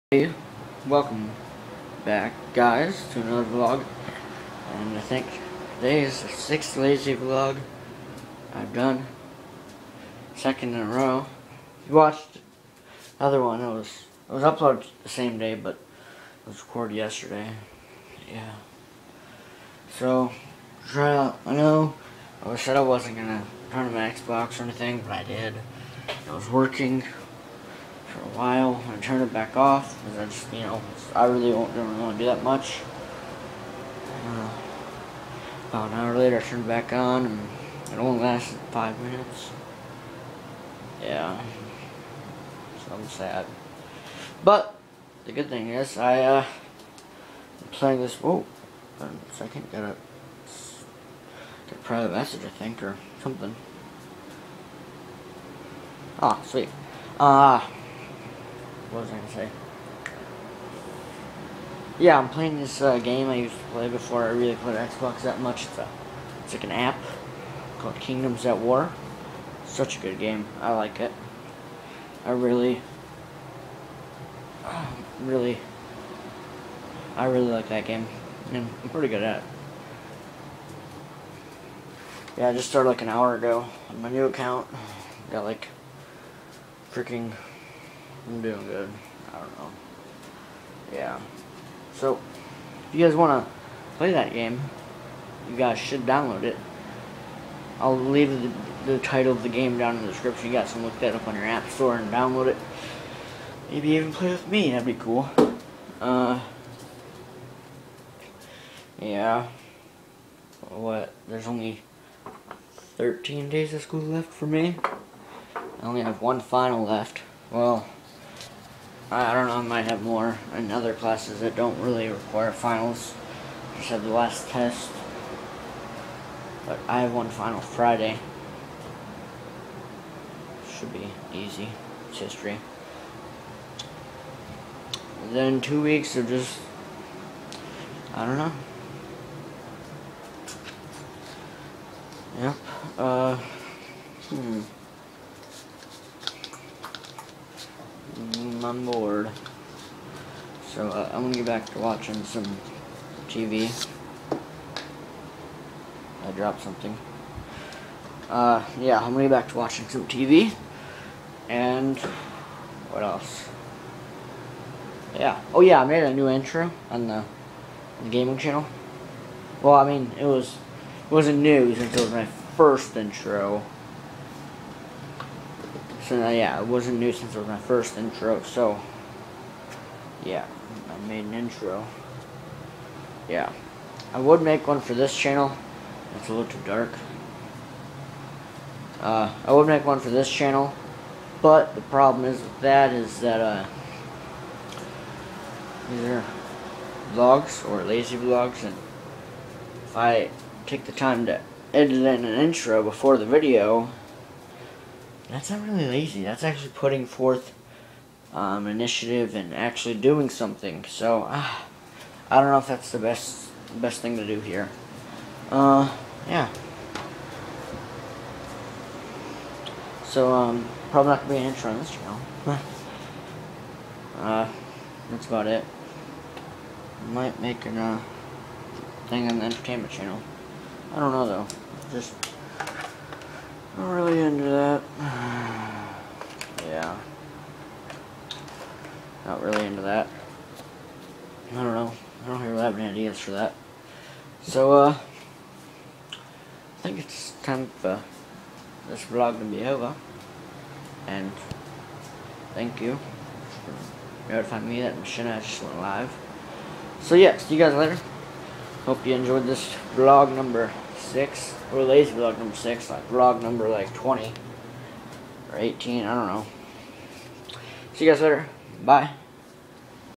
lazy vlog #6 (KingdomsAtWar, Iniative)
The app that I was talking about is called- Kingdoms At War. Look it up and play with me!
My IGN is NalydTheSwordsMaster